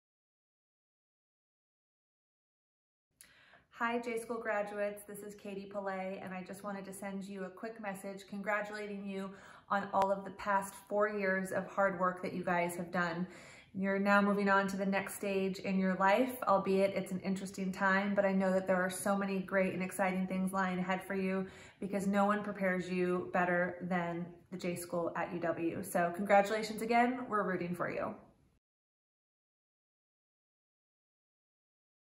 Katie Palay '05 Congratulates UW-Madison School of Journalism and Mass Communication Class of 2020
Principal at A Palay Project for Marketing Consultancy and J-School alum, Katie Palay, shares her message for the Class of 2020.